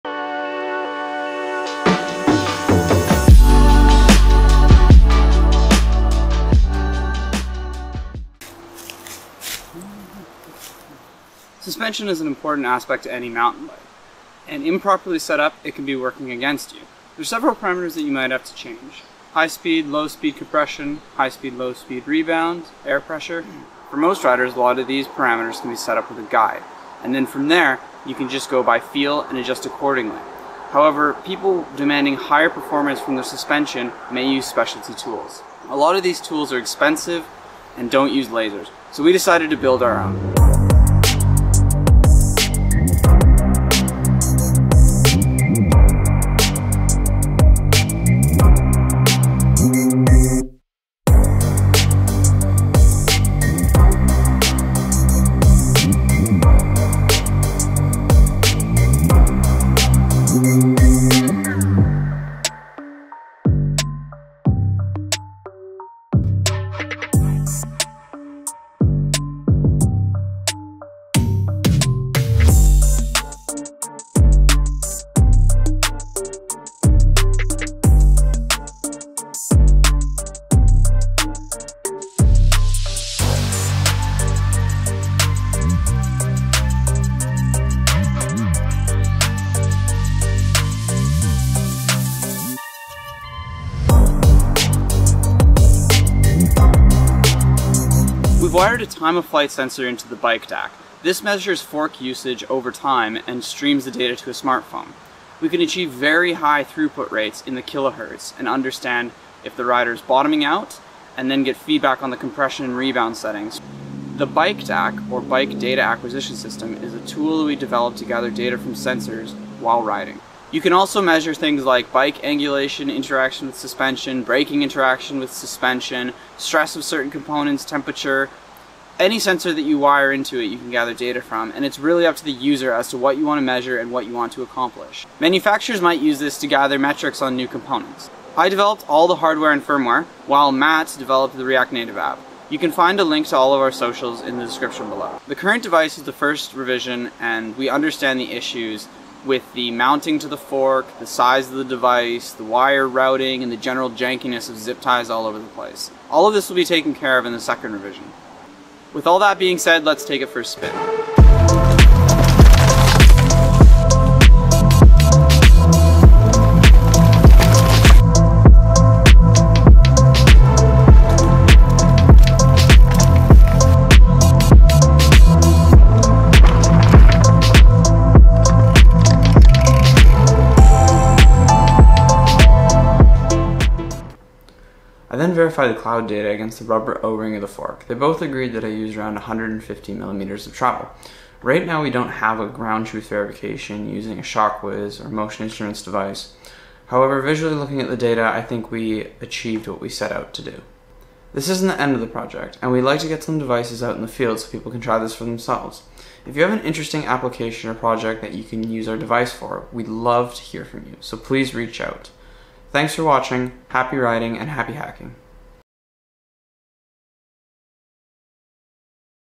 We Measured Mountain Bike Suspension with a LASER! (ShockWiz + Motion Instruments REPLACEMENT?)
In this video, we rigged up the BikeDAQ and laser to a mountain bike to see if we could measure suspension usage.

Follow Matt!